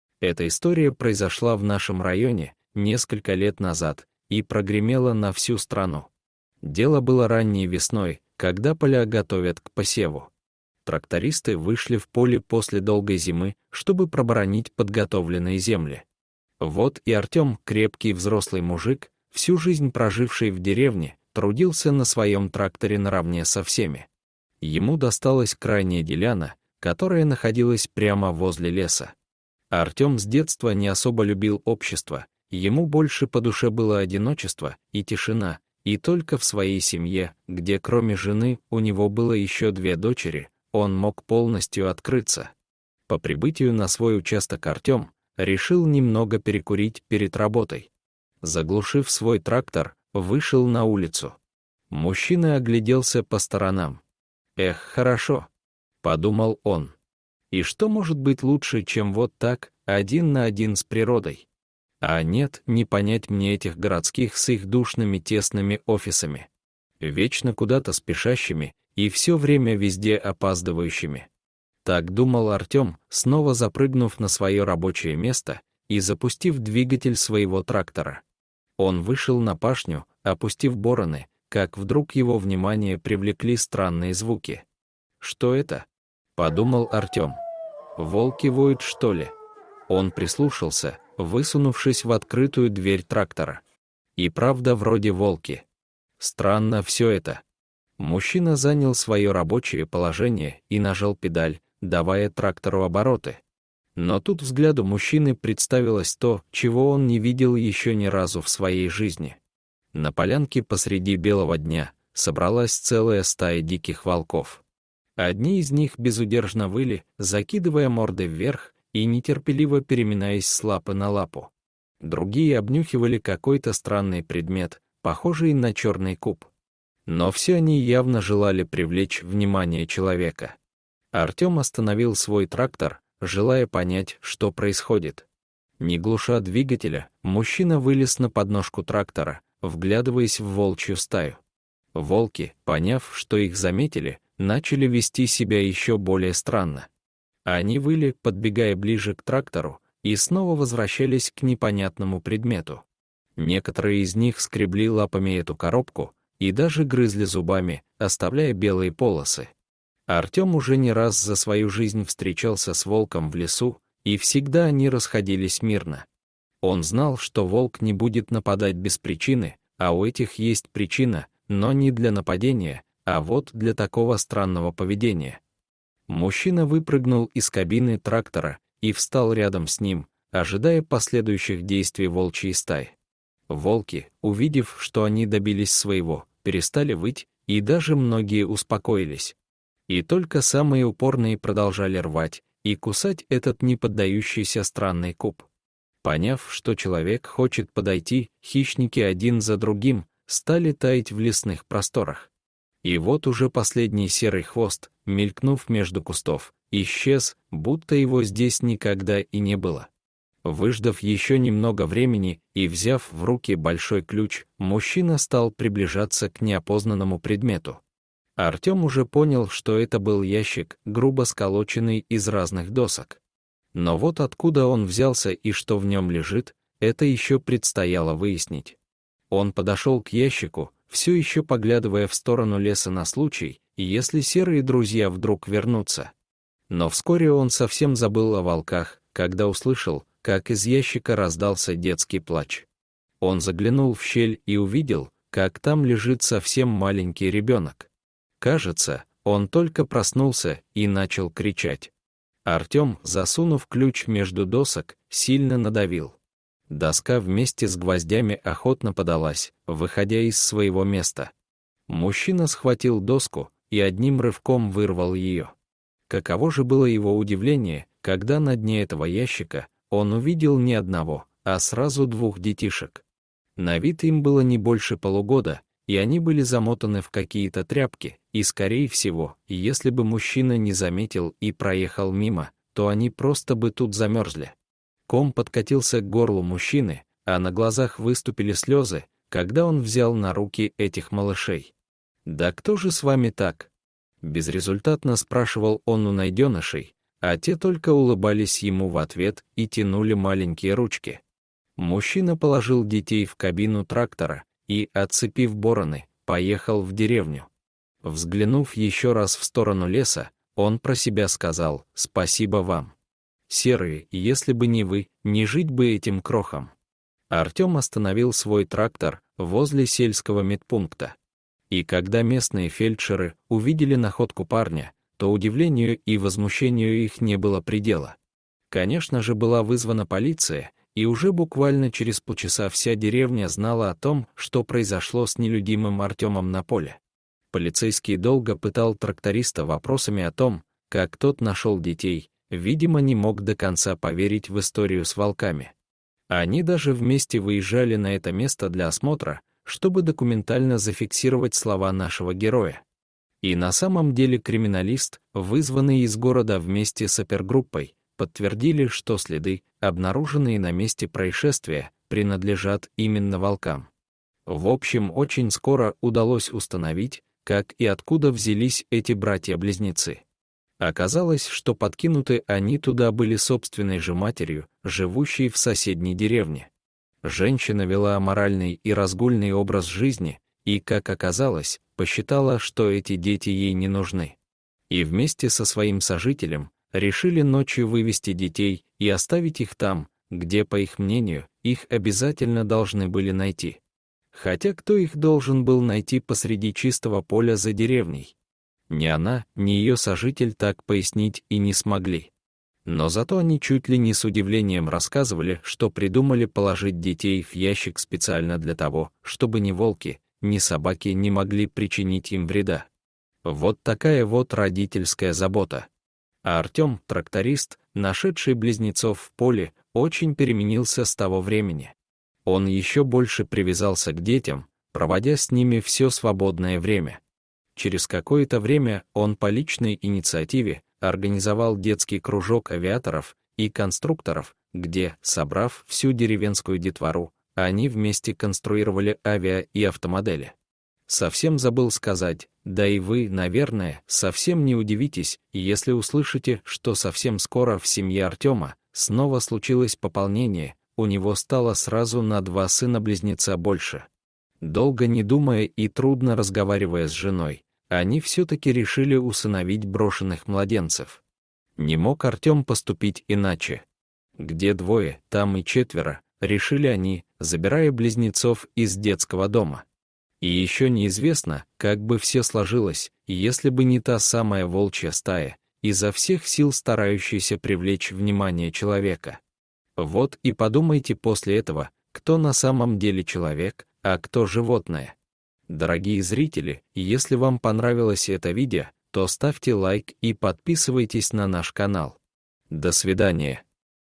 Тракторист увидел стаю волков, окружившую какой-то ящик. Там было ужасное!
Эта история произошла в нашем районе, несколько лет назад, и прогремела на всю страну. Дело было ранней весной, когда поля готовят к посеву. Трактористы вышли в поле после долгой зимы, чтобы проборонить подготовленные земли. Вот и Артём, крепкий взрослый мужик, всю жизнь проживший в деревне, трудился на своем тракторе наравне со всеми.